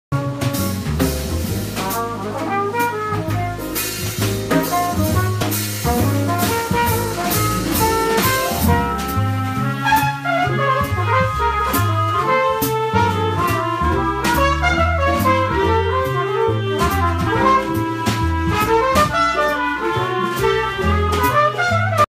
Multitrack recording Jazz @ Lancelot - Asker Jazzclub, Asker, Norway.
Multitrack recording of Erling Wicklund's Storeslem @ Lancelot - Asker Jazzclub, Asker, Norway.